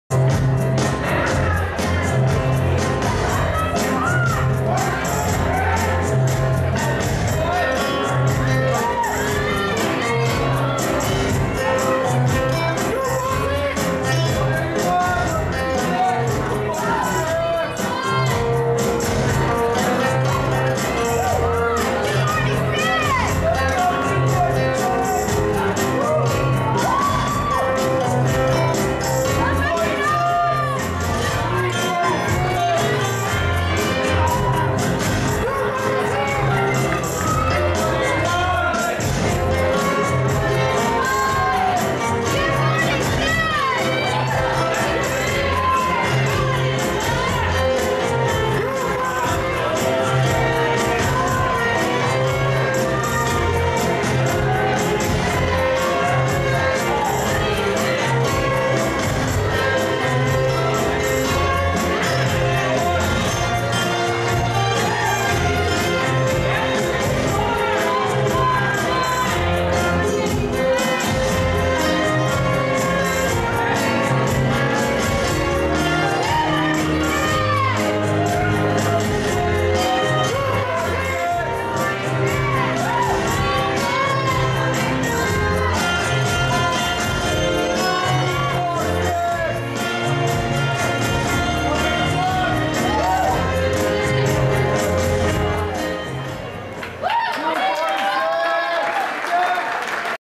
Dance Fest 2014 Bronze Tango Quarter Final
Couples: Sergio Loubriel leading Sarah Strack